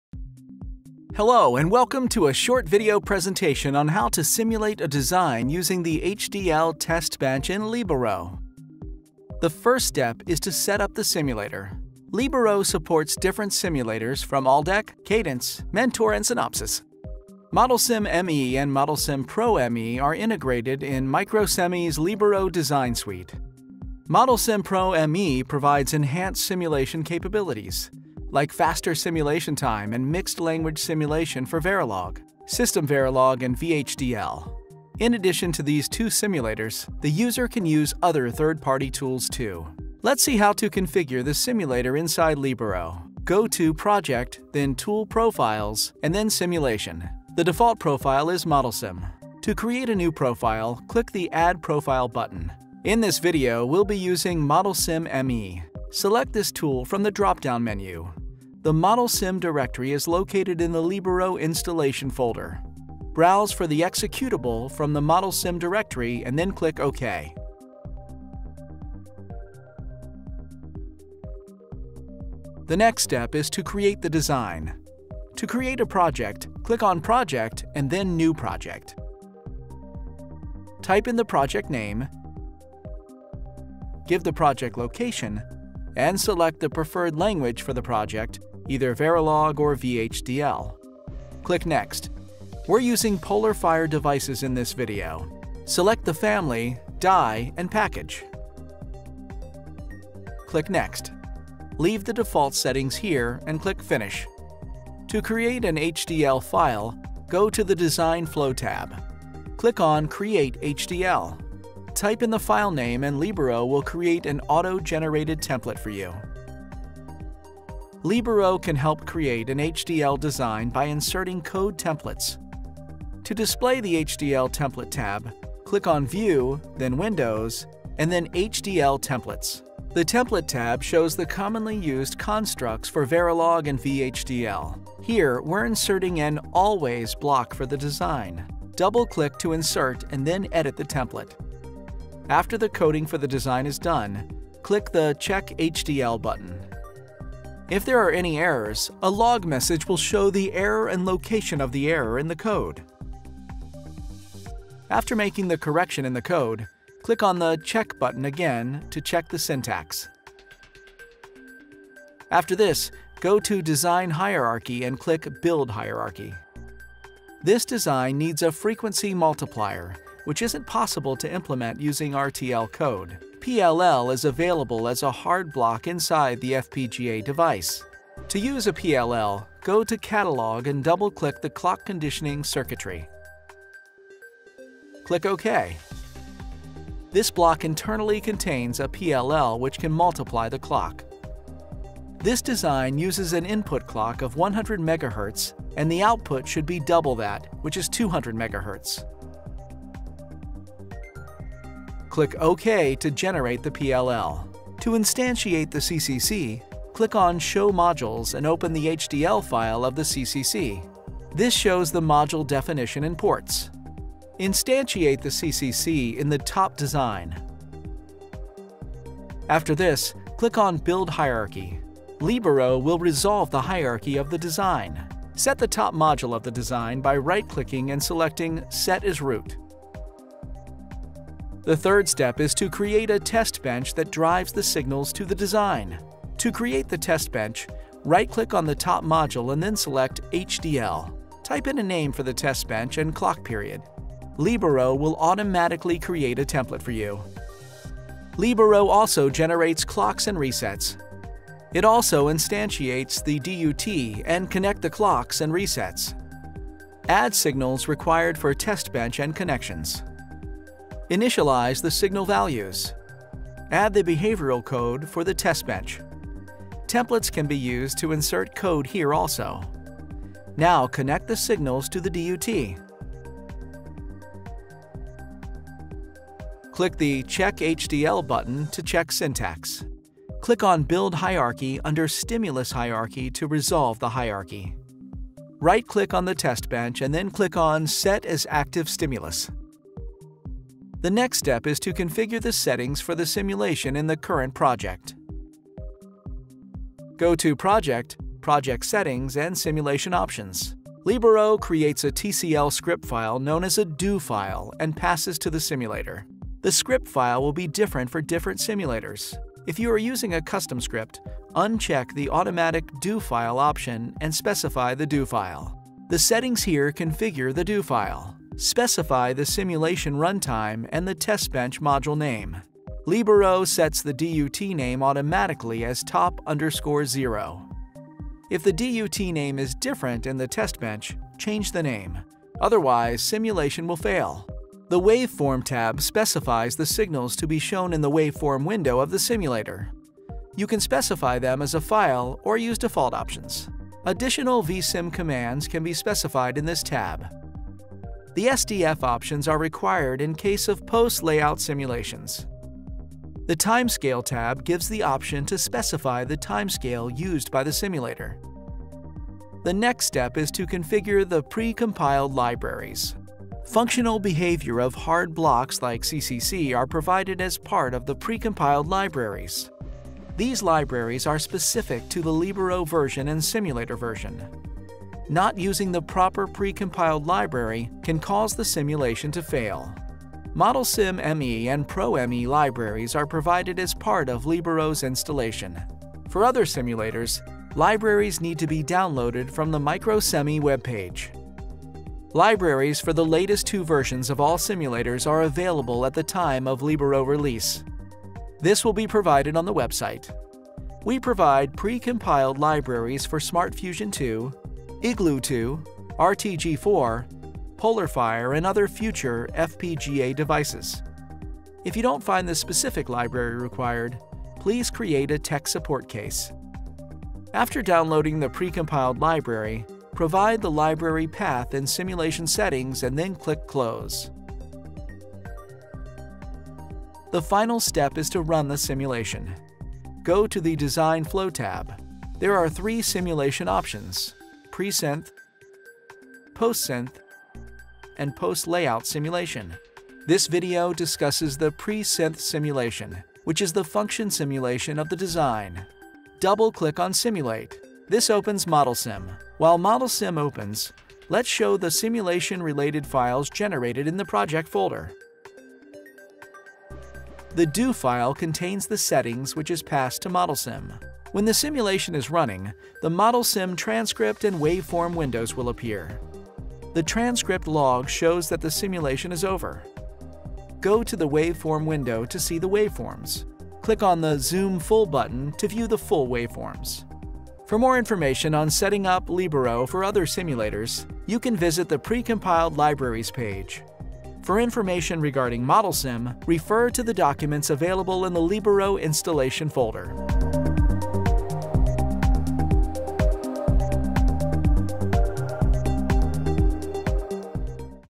How to Simulate Microchip's FPGA Design with HDL Testbench
This video demonstrates the simulation flow using HDL design entry, in which the design and testbench are created using HDL. Libero supports Verilog, System Verilog and VHDL languages.